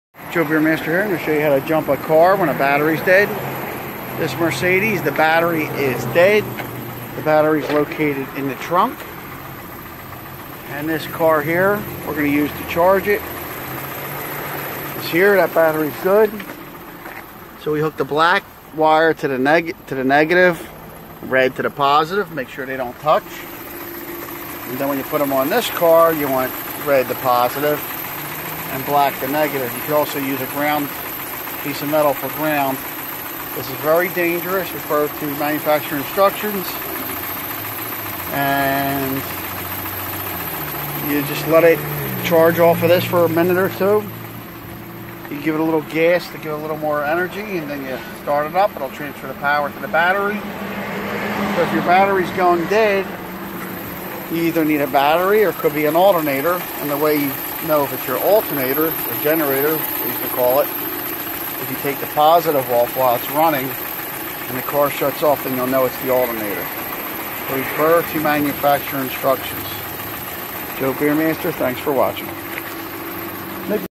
Battery Jump Start From One Car to Another Battery or Alternator
How to jump start from one car to another.. Is it the battery or alternator ?